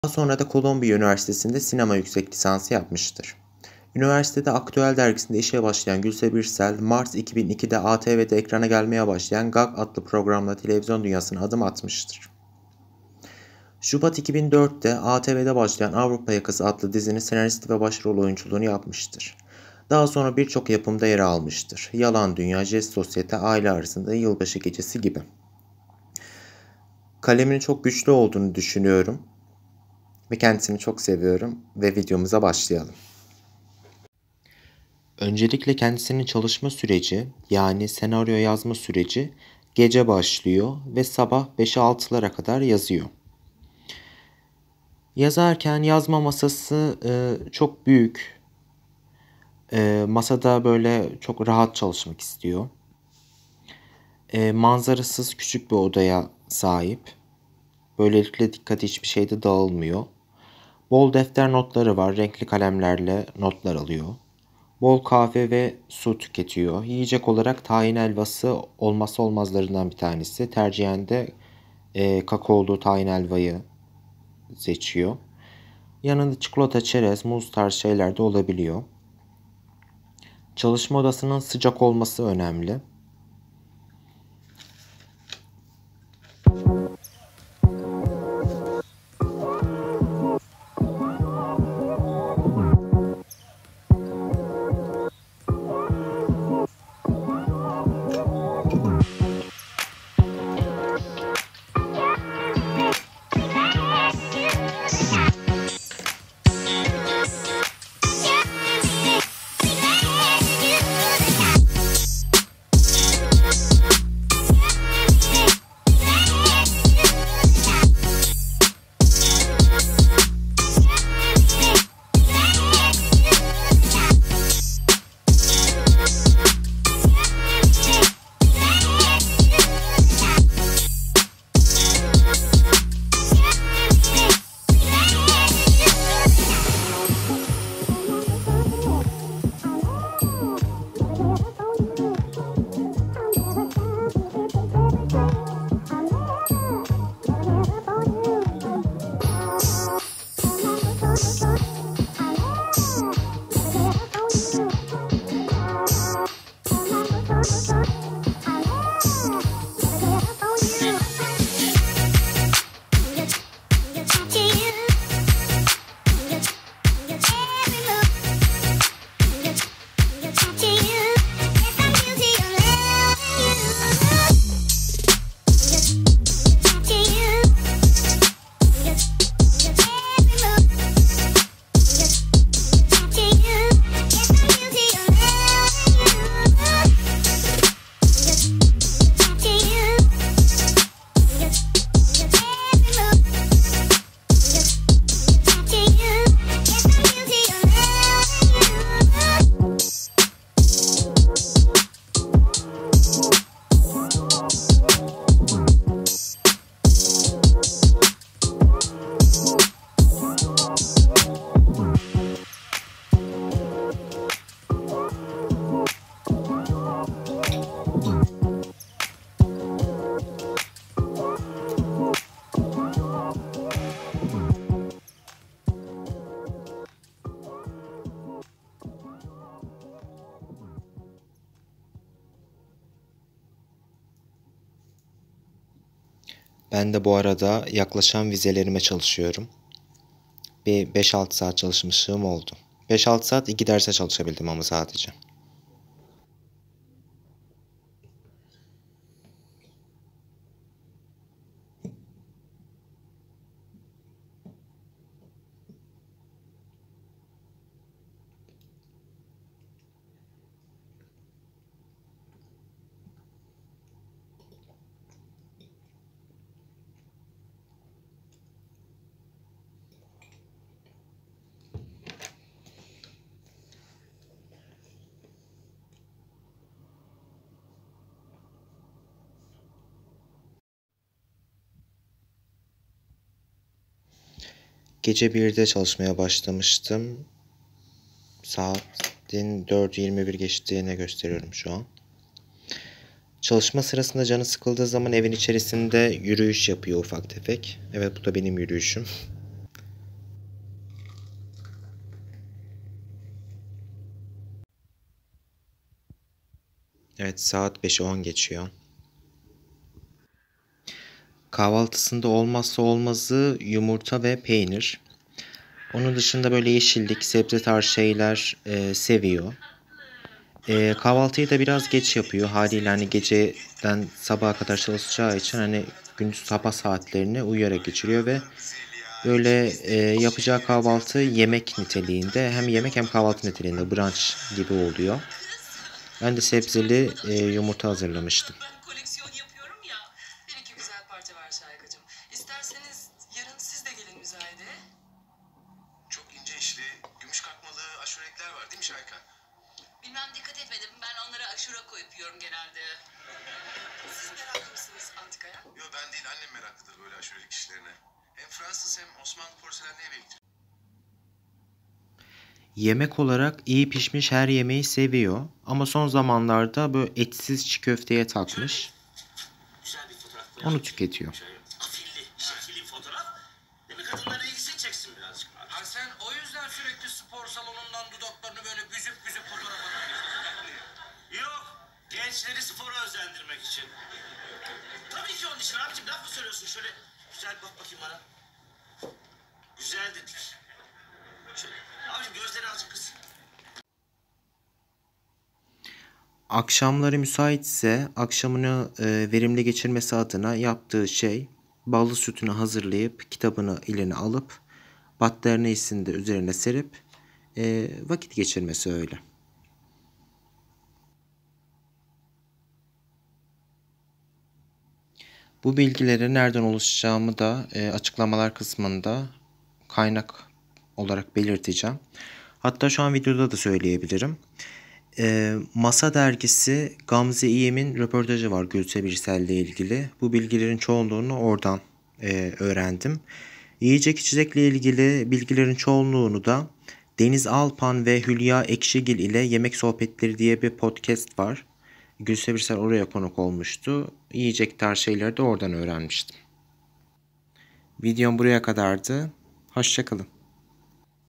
Gülse Birsel'in günlük rutinini deniyorum/💛🧈
Edindiğim Kaynaklar: Masa Dergisi Gamze İyem _ Gülse Birsel röportajı.

Deniz Alphan ve Hülya Ekşigil İle Yemek Sohbetleri Podcast.